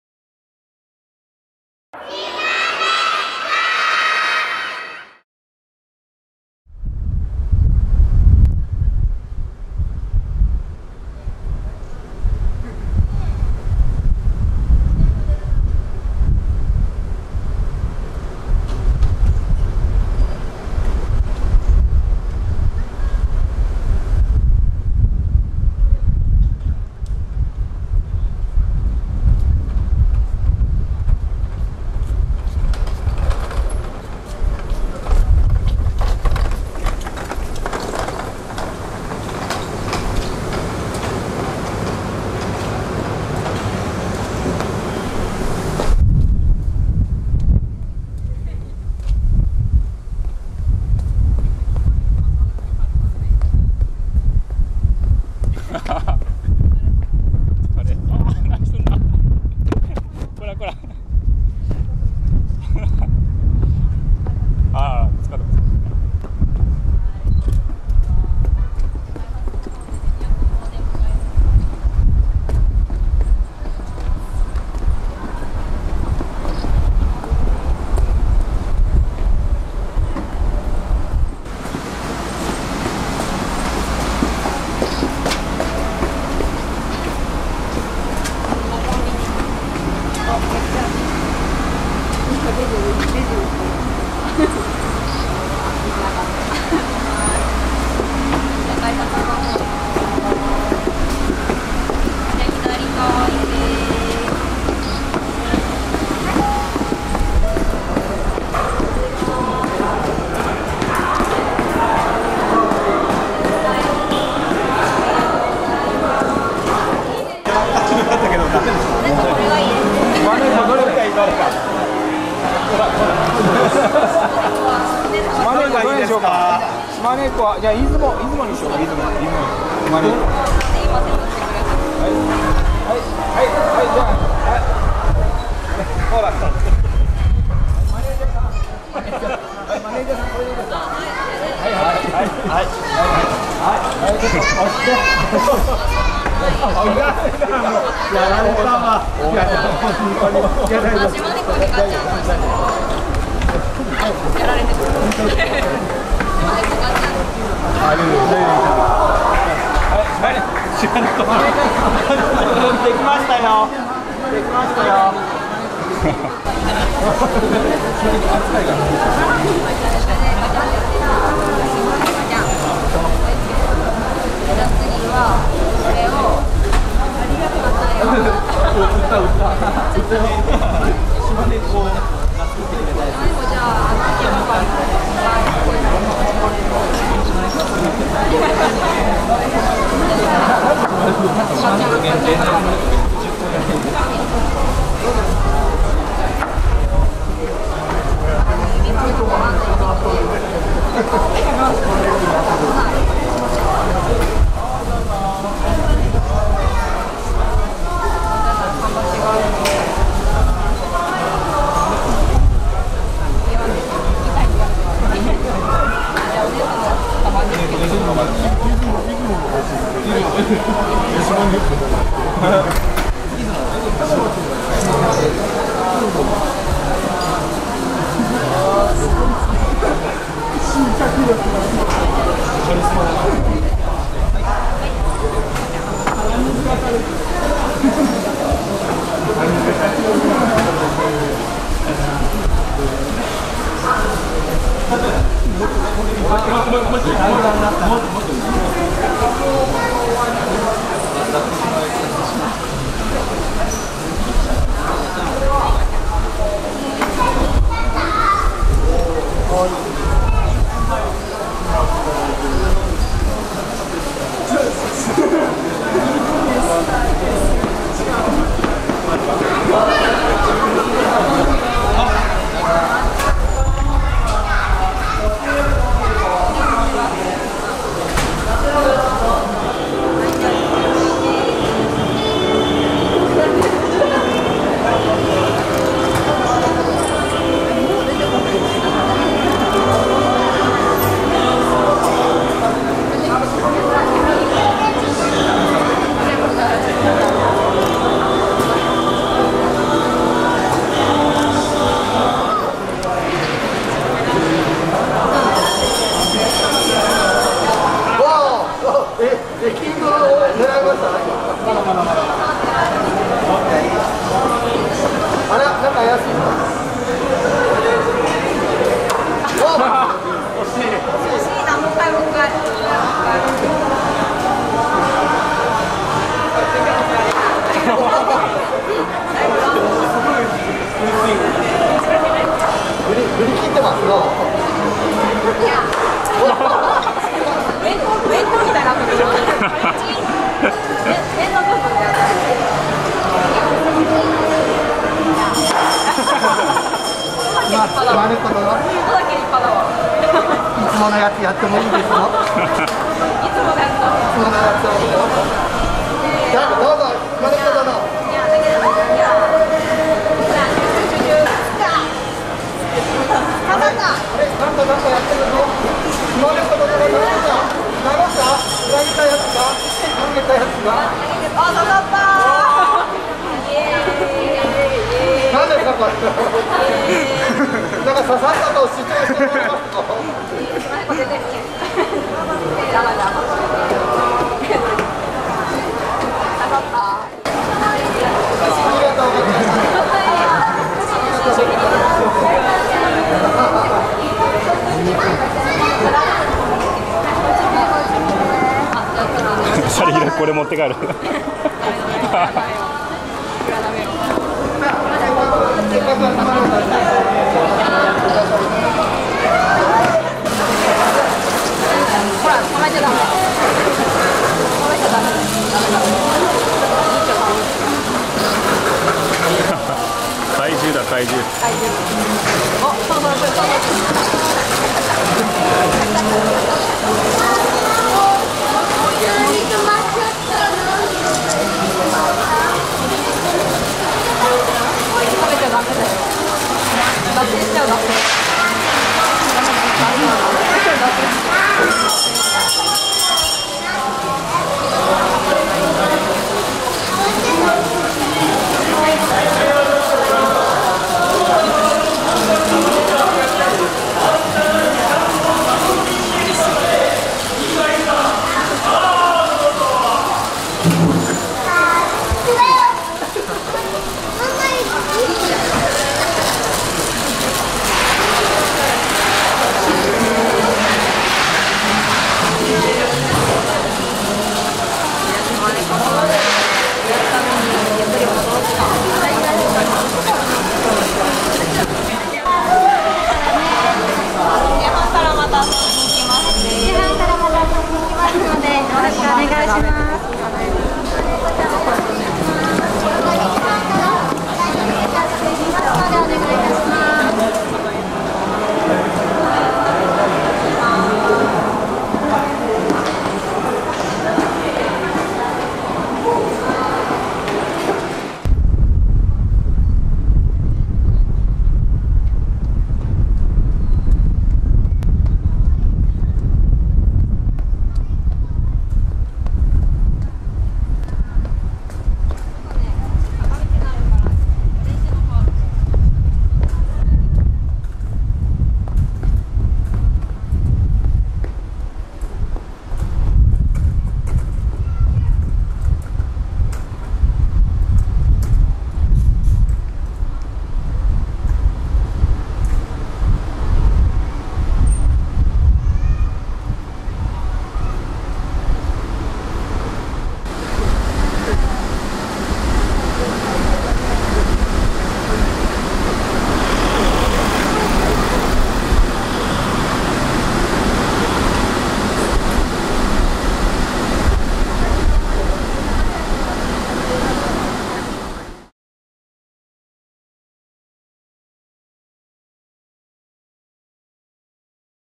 2014/7/19 ② しまねっこ、松江駅まつりへ遊びに行くにゃ！
２回目の登場では、オリジナルバッチ作り（勝手に作られてる）と手裏剣投げで遊んでいました。